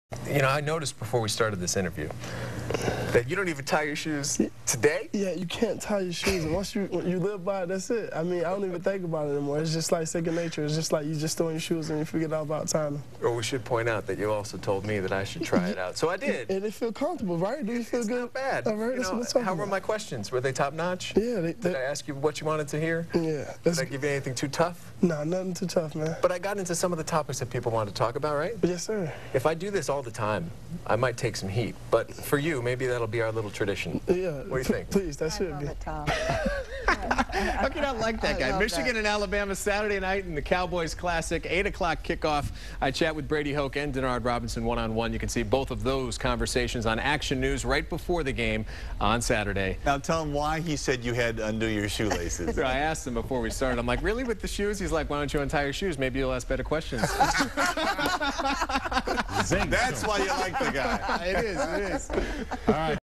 Denard Robinson: 8/29/12
Denard Robinson told Tom Leyden to try untying his shoes - he might ask better questions.